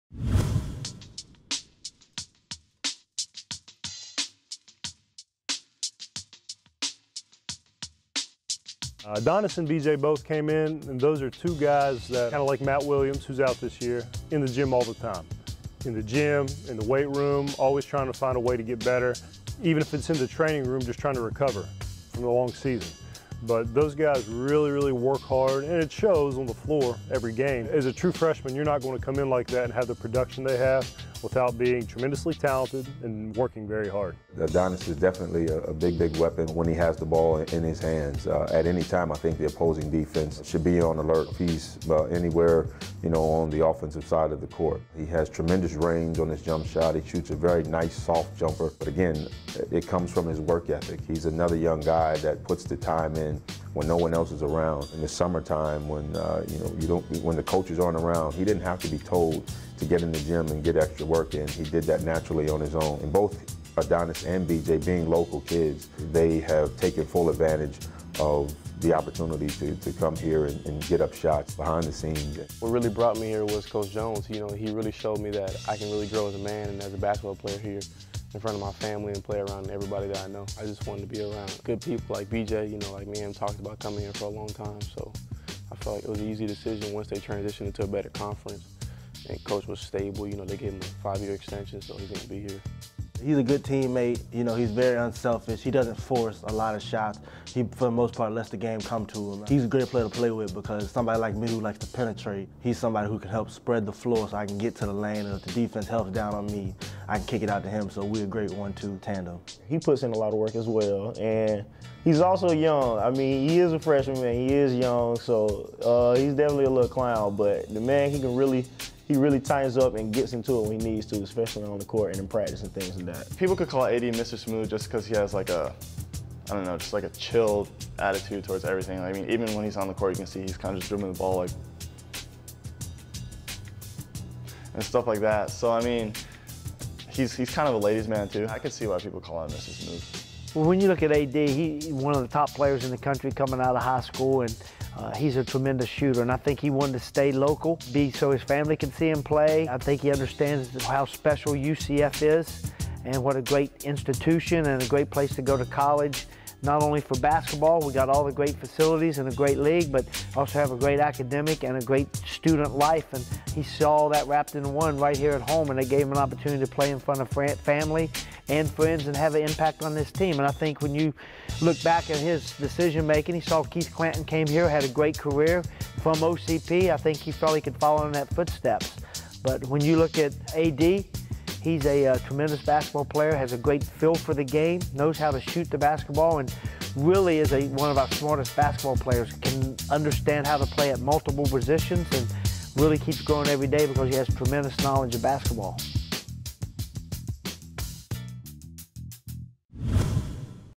IUCFB Player Profile 2015: Adonys Henriquez, Freshman Guard
Local product Adonys Henriquez is one of the first guys on the court for practice and often turns out the lights as the last to leave.  He's part of the talented true freshmen backcourt for the Knights that has fans excited for the future.

Featuring interviews with:  Adam Williams, Asst. Dir. of Basketball Operations; Craig Brown, Dir. of Basketball Operations; BJ Taylor, freshman guard; Myles Davis, senior guard; Dylan Karell, junior forward; Shawn Finney, Assoc. Head Coach;